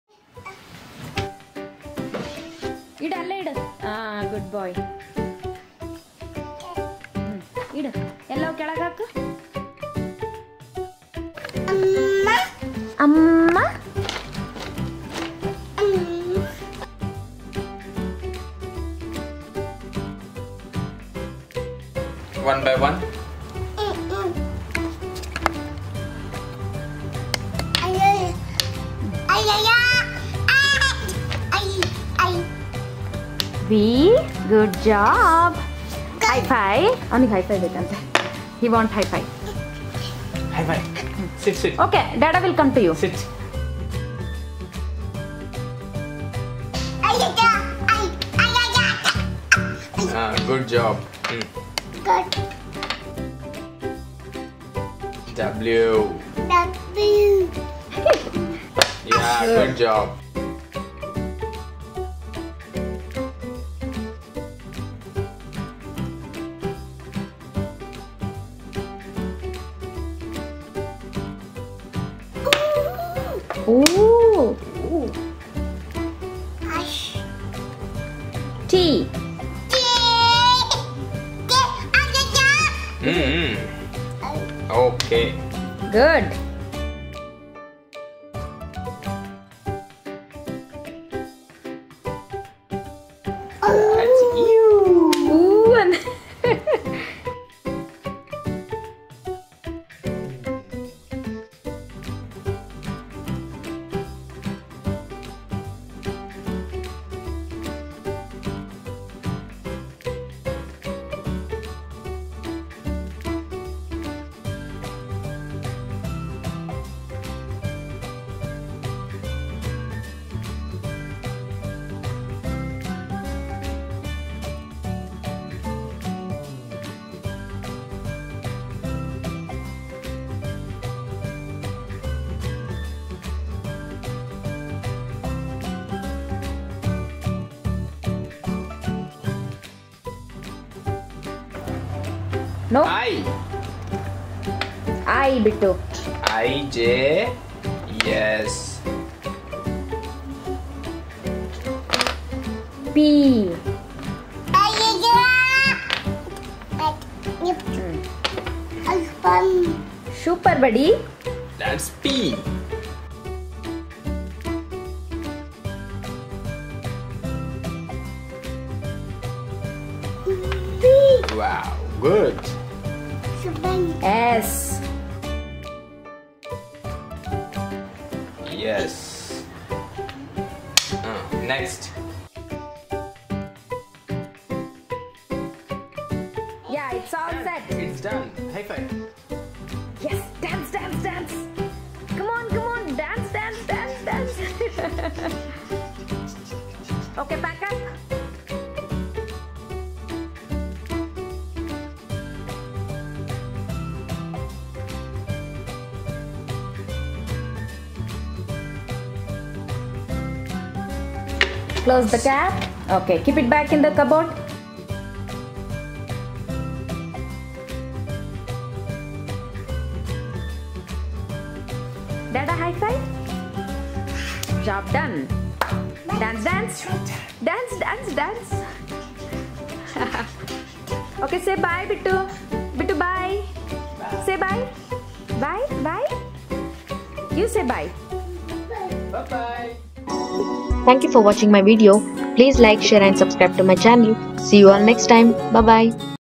1year baby solving alphabet puzzle || ABC learn n play
Let's be more connected to each other